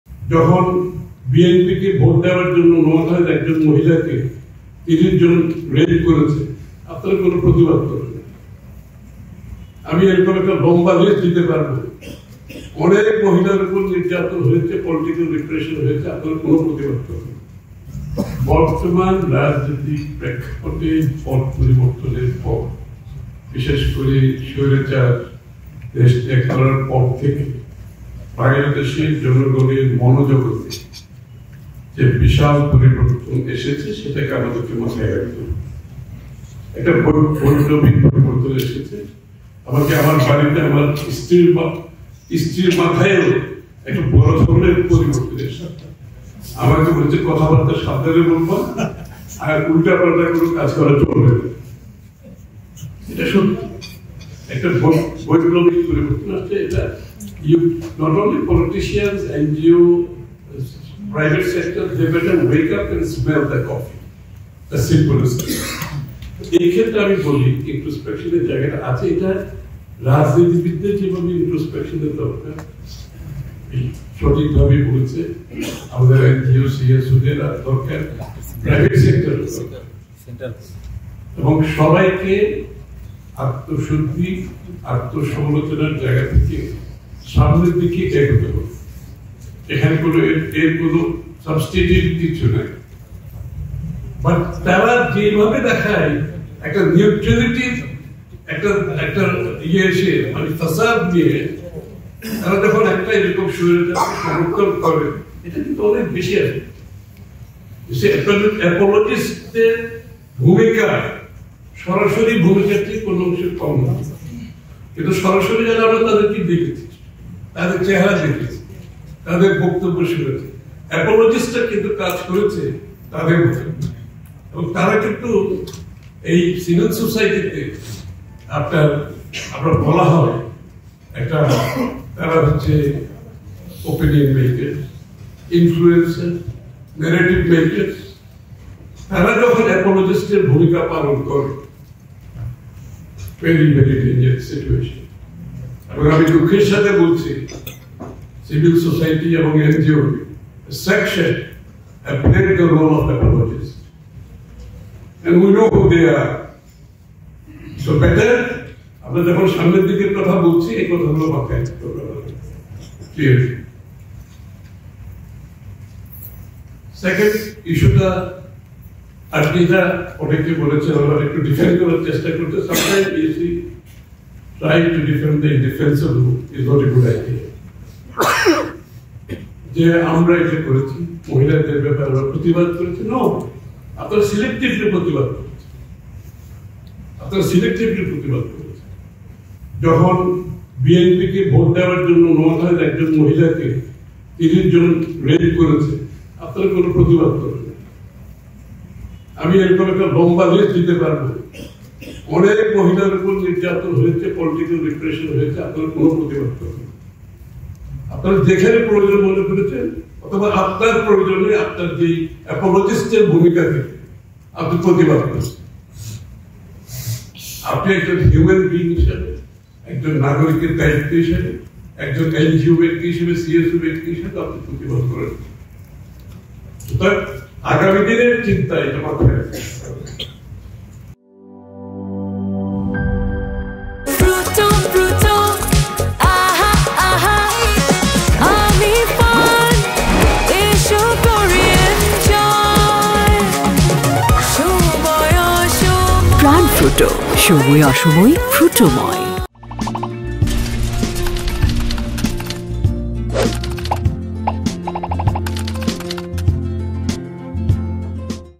বিএনপিকে ভোট দেওয়ায় নারীকে ধ-র্ষ-ণে-র ঘটনায় এনজিও চুপ ছিল | Jago News
আওয়ামী লীগ সরকারের আমলে নারীরা নি-র্যা-তি-ত হলেও নারী অধিকার নিয়ে কাজ করা এনজিওগুলো বেছে বেছে প্রতিবাদ করেছে বলে মন্তব্য করেছেন বিএনপির স্থায়ী কমিটির সদস্য আমীর খসরু মাহমুদ চৌধুরী।